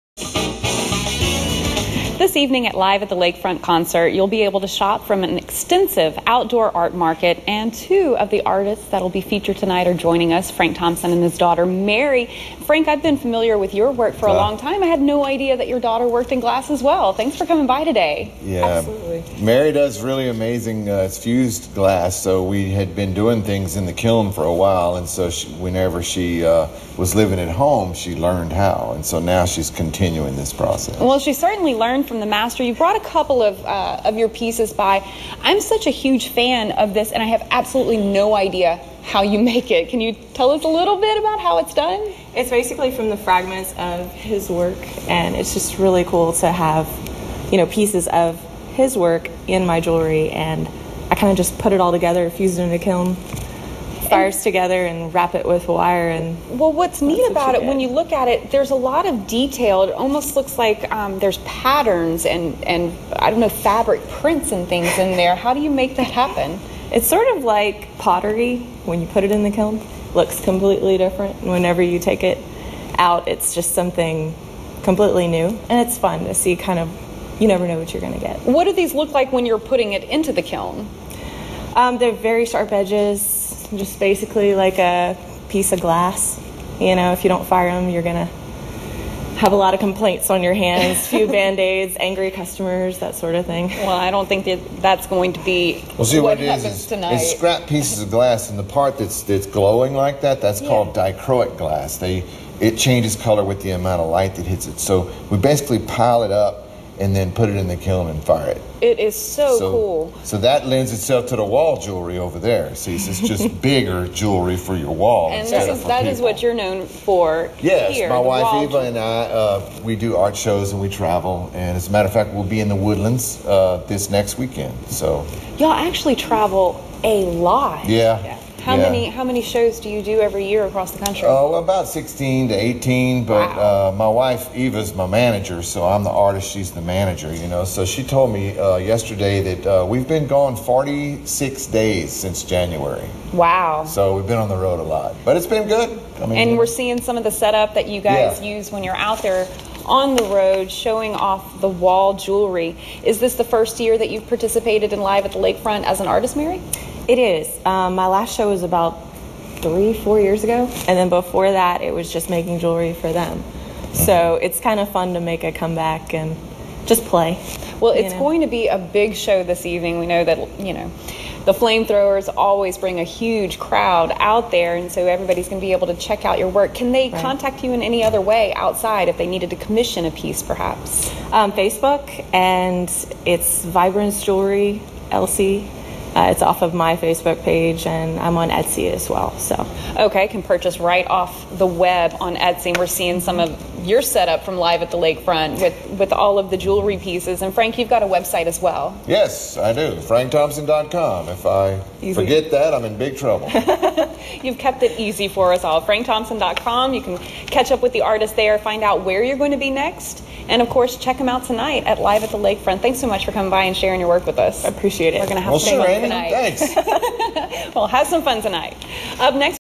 FATHER-DAUGHTER ARTIST DUO
FRANK THOMPSON AND DAUGHTER MARY THOMPSON WILLIAMS WILL BE AT LIVE AT THE LAKEFRONT THIS EVENING!